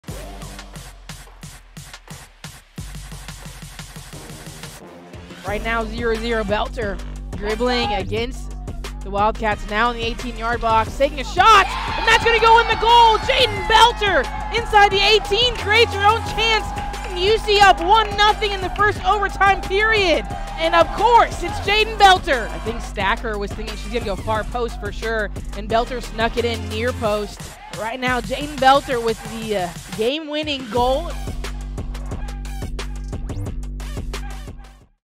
University of the Cumberlands - Pepsi Play of the Week - Jayden Boelter 11-21-2022
Pepsi Play of the Week for November 21st, 2022.  #3 Cumberlands Women's Soccer in the NAIA Second Round.  The Patriots get an extra-time goal from Jayden Boelter in their 1-0 win over Indiana Wesleyan University on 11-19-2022, securing a spot at the NAIA final site.